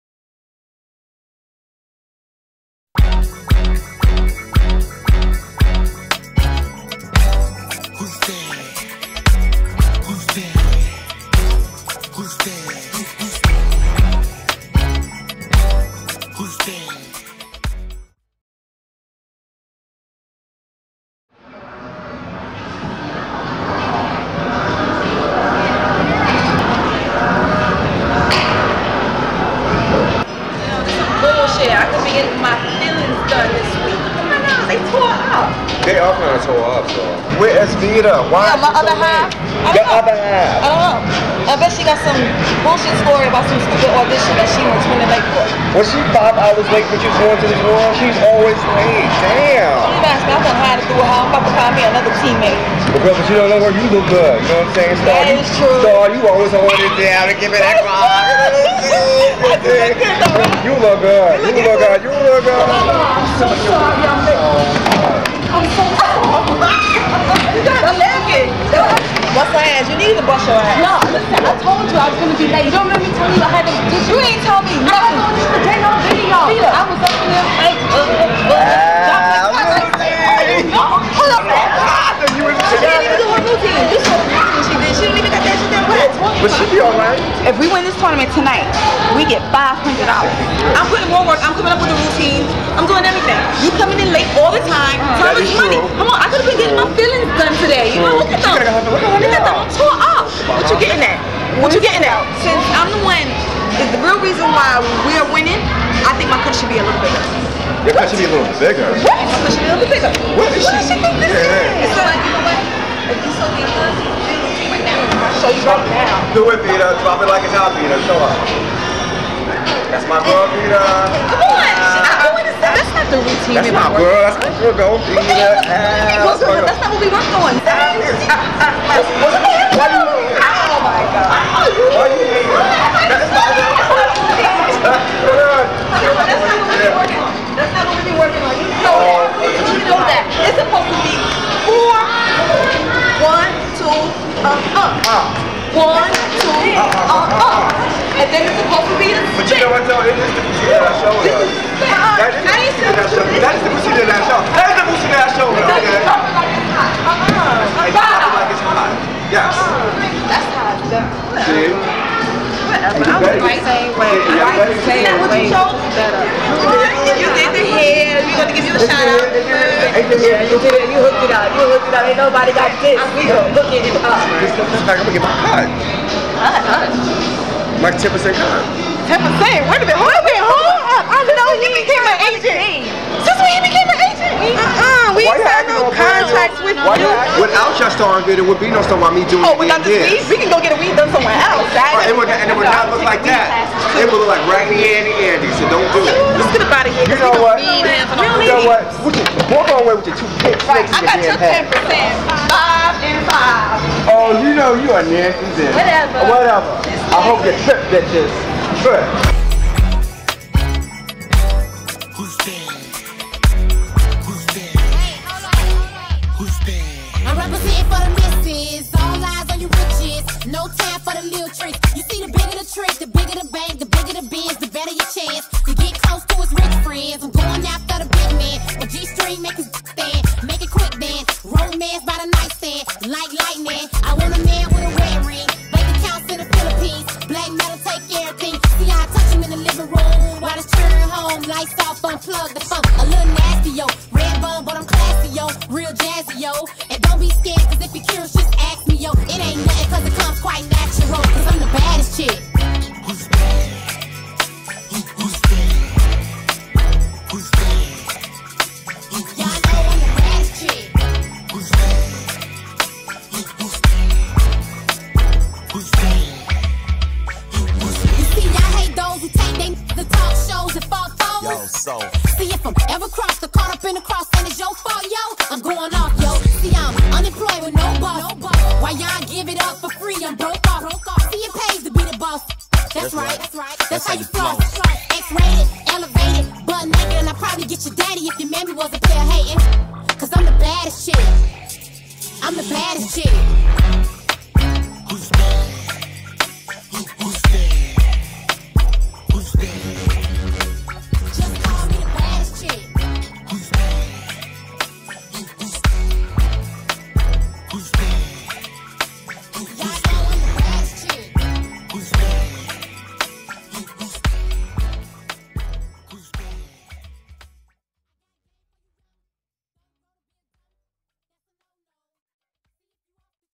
Empire Skating Rink
Filmed in Brooklyn's historic Empire Skating Rink. Greed threatens to unhinge a freeform skate team when they start to argue over prize money for a dance contest - before they've competed.